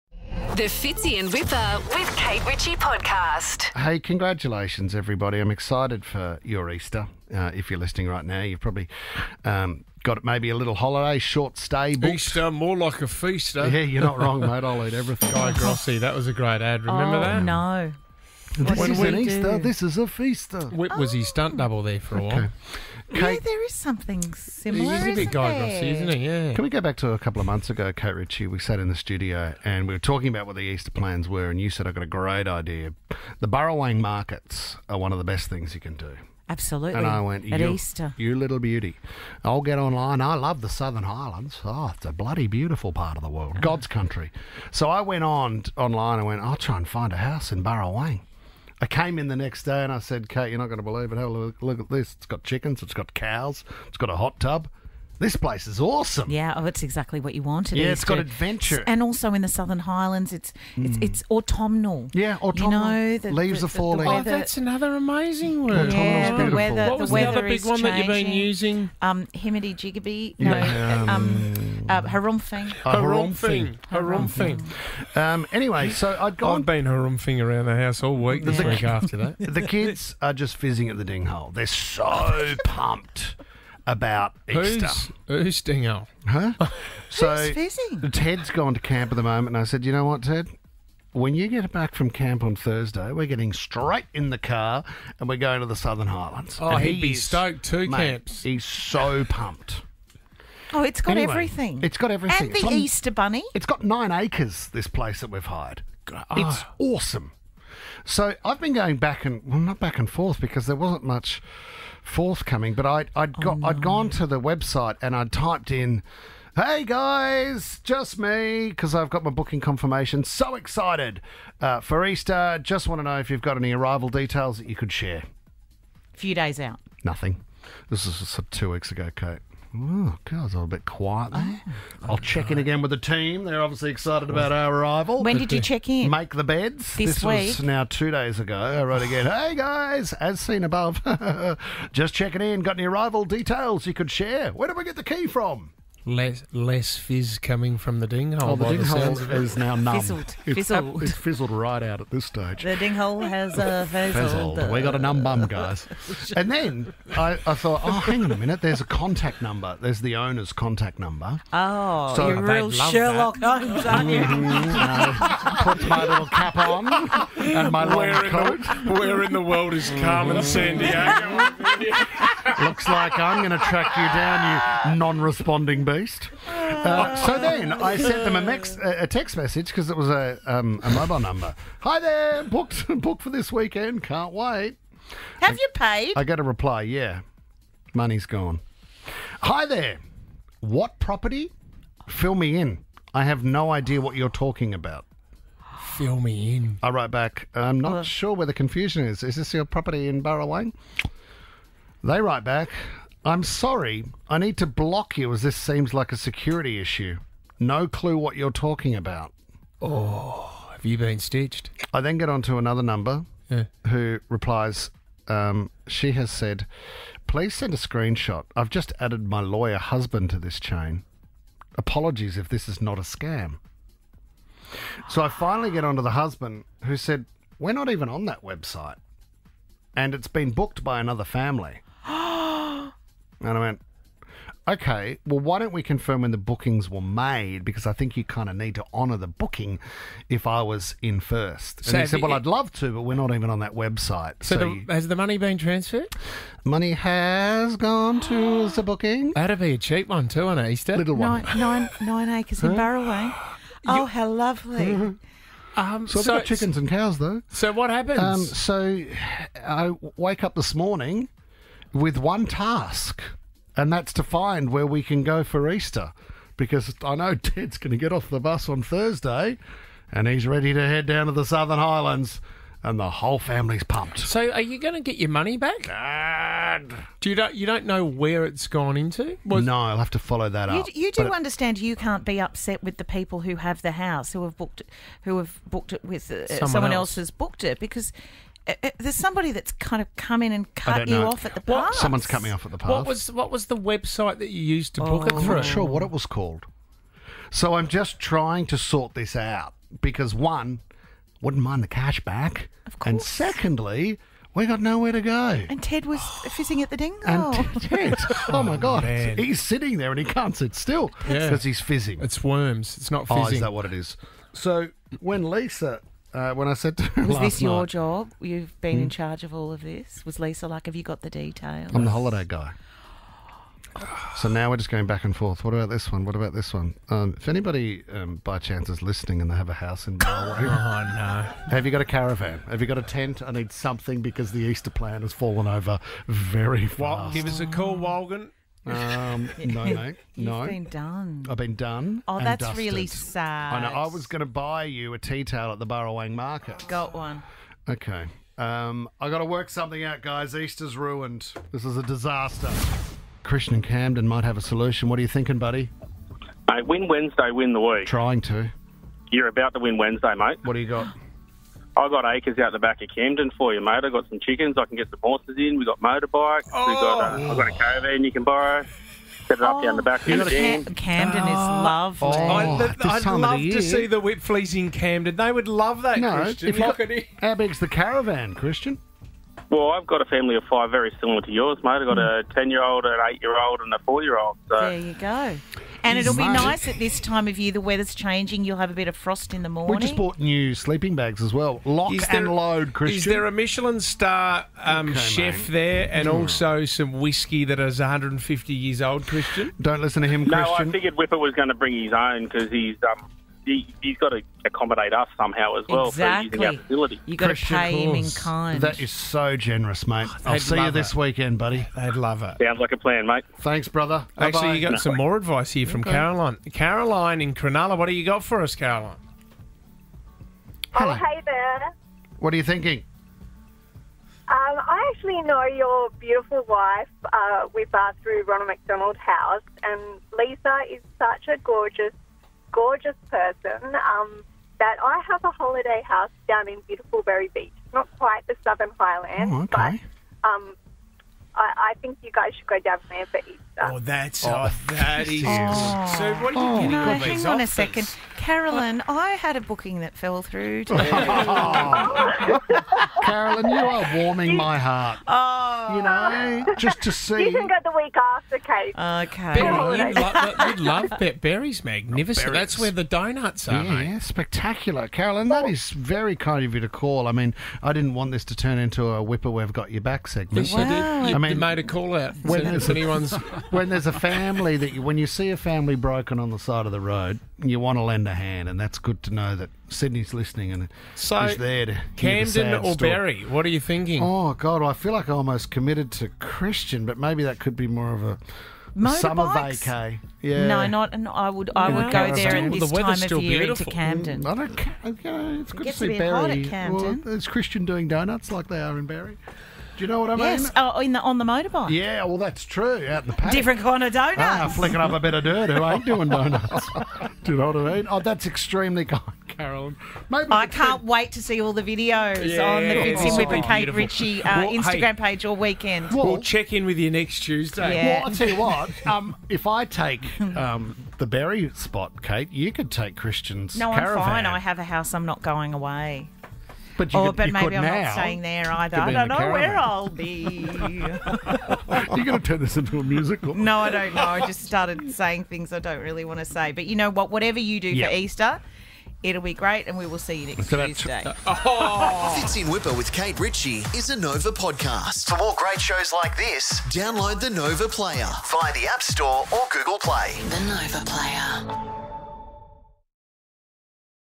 Wippa’s F****d Up Easter For His Family
Wippa’s family has been fizzing at the dinghole for their Easter family holiday to the Southern Highlands. It appears Wip’s stuffed up big time with booking the accom and is now in desperate need of somewhere to spend the Easter hols before his family loses fizzing momentum.